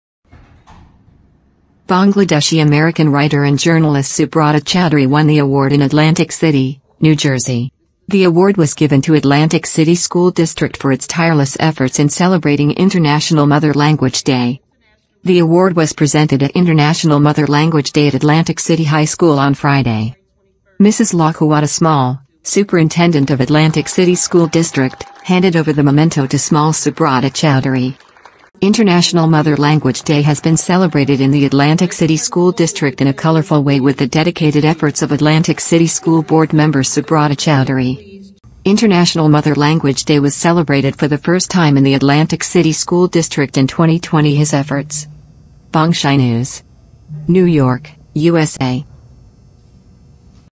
Journalist Subrata Chowdhury Was Awarded
Bangladeshi-American writer and journalist Subrata Chowdhury won the award in Atlantic City, New Jersey. The award was given to Atlantic City School District for its tireless efforts in celebrating International Mother Language Day.
The award was presented at International Mother Language Day at Atlantic City High School on Friday.

Mrs. La Quetta Small, Superintendent of Atlantic City School District, handed over the memento to Small Subrata Chowdhury.

International Mother Language Day has been celebrated in the Atlantic City School District in a colorful way with the dedicated efforts of Atlantic City School Board member Subrata Chowdhury.

International Mother Language Day was celebrated for the first time in the Atlantic City School District in 2020 his efforts.